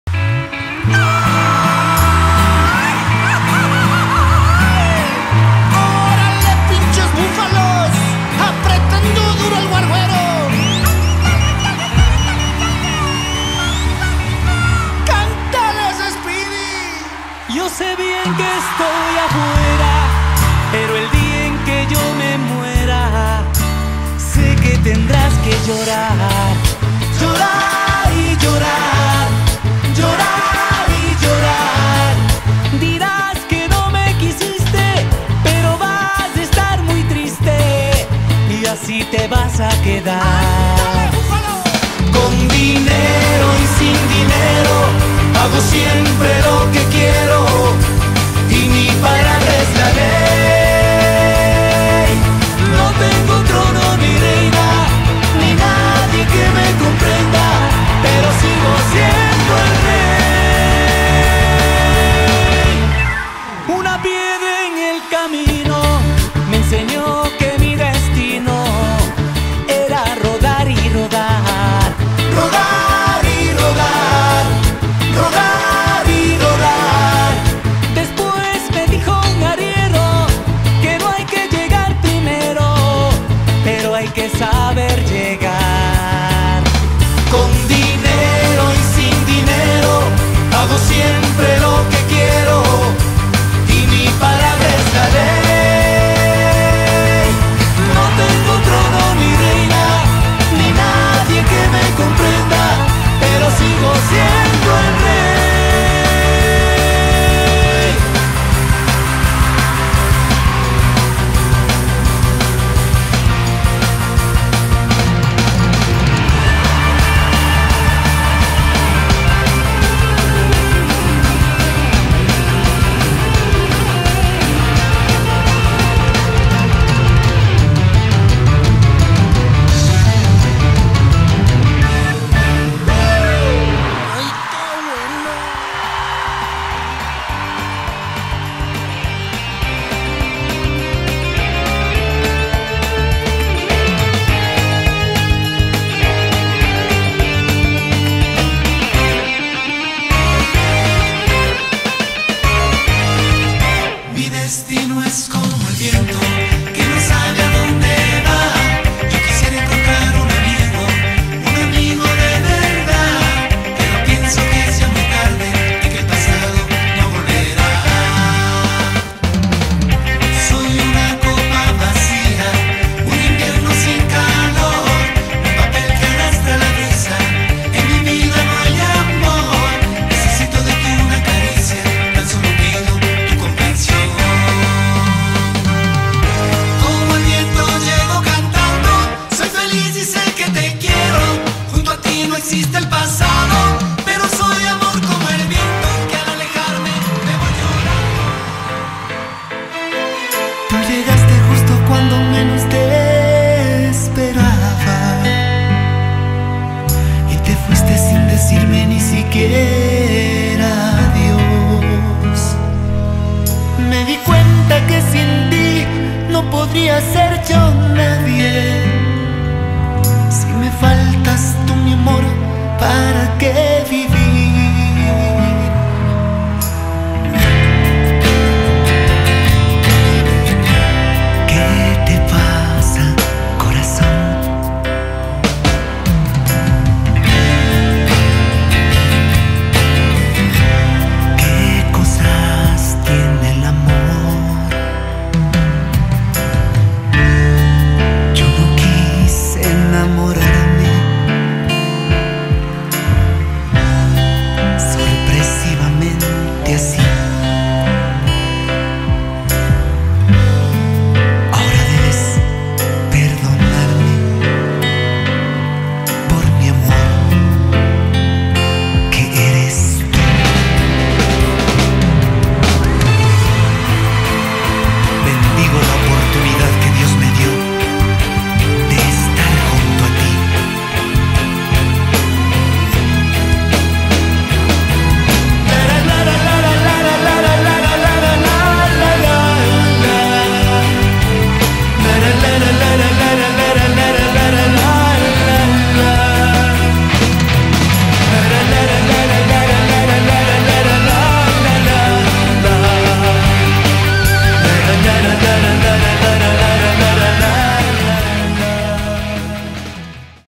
Los Bufalos - Carnavales Paute
CONTRATOS : Movistar:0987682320 Claro: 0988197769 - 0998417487
GIRA 2024🎤🎹🎸
Les compartimos lo que fue nuestro segundo día de carnavales por Paute, gracias a todos los que se dieron cita en tan hermoso concierto.
LA HISTORIA DEL AMOR, CONTINÚA
PORQUE EN EL 2024 son 26 AÑOS.